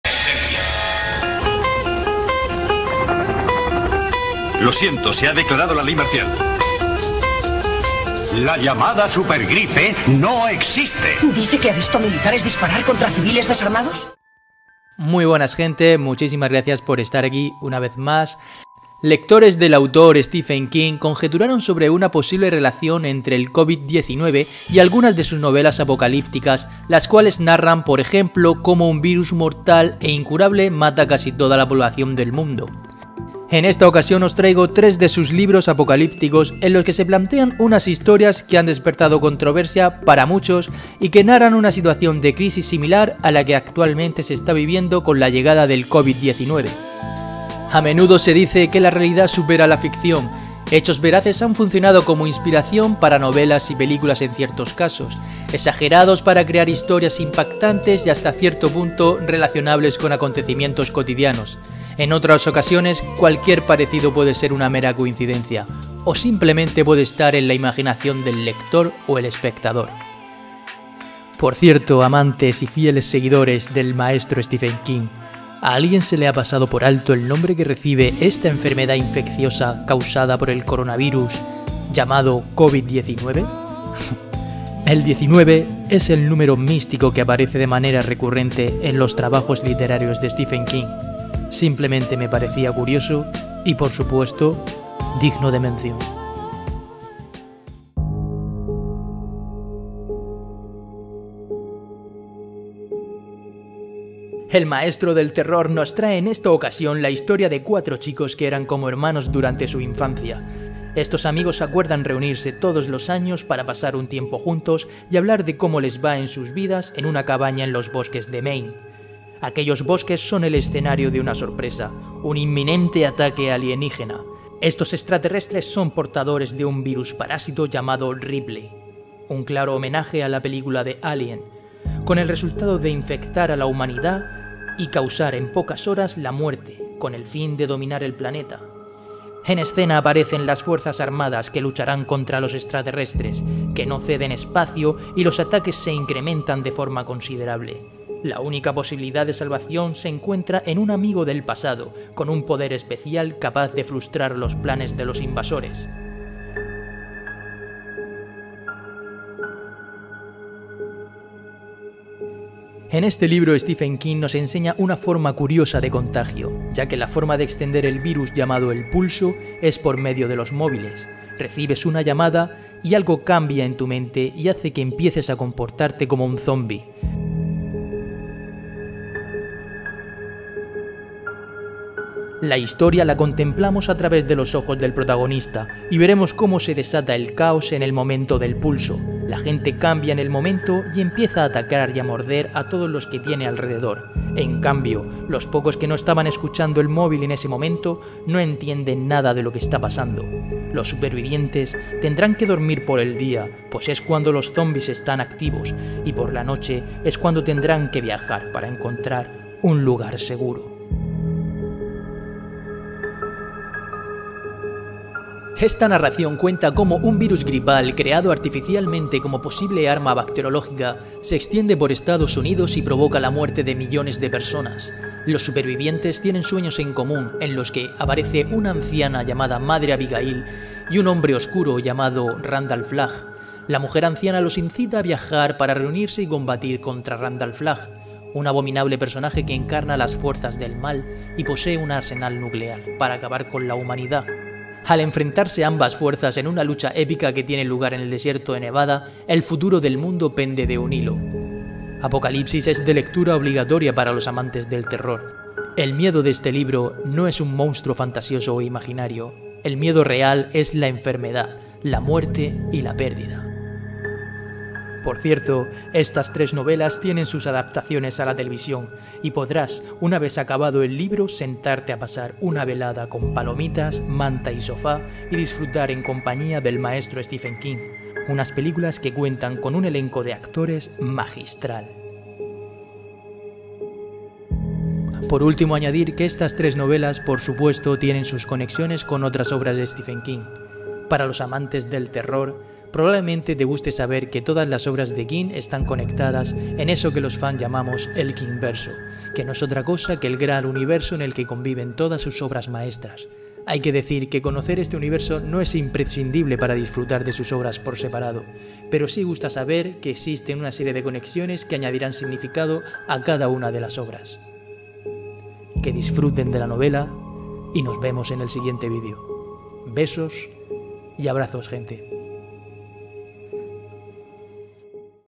😱3 Libros APOCALÍPTICOS de STEPHEN KING 😱 que NO PODRÁS dejar de leer
La obsesión por el coronavirus no ha hecho más que comenzar. Los más aprensivos llevan semanas buscando ejemplos cinéfilos o seriéfilos que muestren cómo podría afectar el nuevo virus al planeta Tierra.

Han sido muchos los que han sacado parecido con Apocalipsis (The Stand, en su versión original) de Stephen King, que fue adaptada como miniserie en 1994 y tiene pendiente este mismo año una nueva versión.

Hay que recordar que Apocalipsis narra la historia de un virus altamente mortal, que hace que el mundo sufra un verdadero apocalipsis. Una historia que comienza con el escape y la propagación del arma biológica, conocida como ‘el proyecto azul’, y coloquialmente como ‘el capitán trotamundos’, que consigue trasladarse por todo el mundo, y tiene una tasa de infección y de mortalidad superior al 99%.

LIBROS:
El cazador de sueños